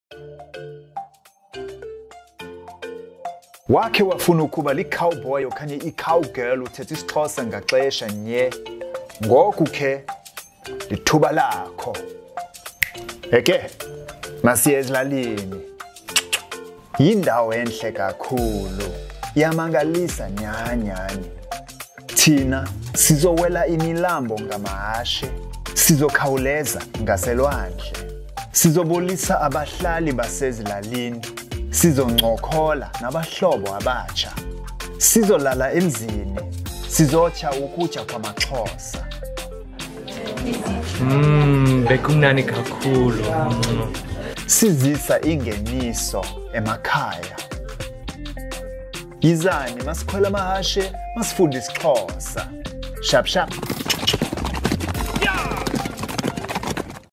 Can you learn Xhosa on horseback?
Want to learn Xhosa on Horseback on one of the most beautiful and wild coastlines in the world?

Partnering with Terrakhaya's natural horsemanship, bit-less trained horses for riders of all levels, on the WildCoast Horse-ride of a lifetime (some basic horse riding experience required).

DATES:   9th-16th June 2022

From Mdumbi to Port St Johns, Eastern Cape, South Africa’s Wild Coast.

Only 5 places available!